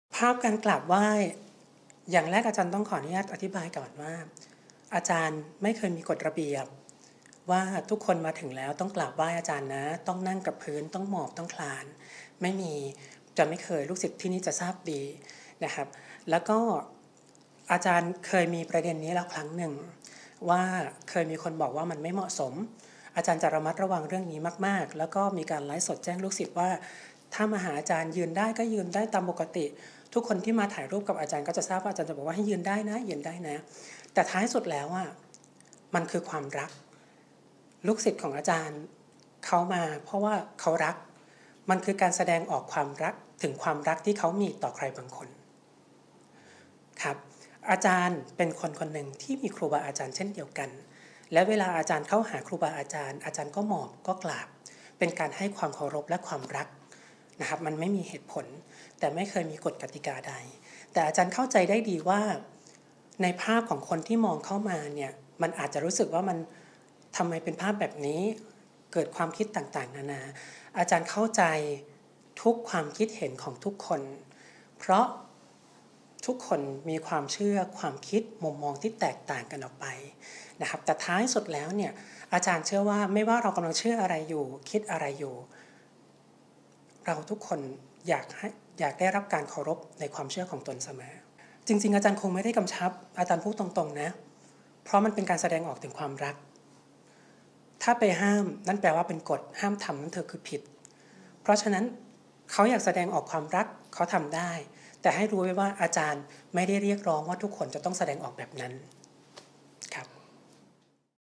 “อาจารย์เอ จักรพรรดิ” แจงดรามาคลิปลูกศิษย์ก้มหมอบกราบ เชื่อรักศรัทธา
เปิดประวัติ ทำความรู้จัก “อาจารย์เอ จักรพรรดิ” กว่าจะมีวันนี้ เส้นทางไม่สวยหรู เคยผิดพลาดธุรกิจ ล้มละลายมาก่อน หลังบนบานศาลกล่าวเกิดปาฏิหาริย์ ลั่นวาจาขอเป็นตัวแทนของเทพไท้เทวาครูบาอาจารย์ในการสื่อสารเรื่องศรัทธากับผู้คน เตรียมสร้างสถานปฏิบัติธรรมและสถานที่สอนสัมมาชีพ พร้อมแจงประเด็นดราม่า ลูกศิษย์ลูกหา ก้มหมอบกราบเกิดจากความรักความที่มีต่ออาจารย์ ด้านลูกศิษย์กลั้นน้ำตาไม่อยู่ เล่าวินาทีชีวิตดำดิ่งแต่หลังจากมาเจออาจารย์แล้วชีวิตดีขึ้น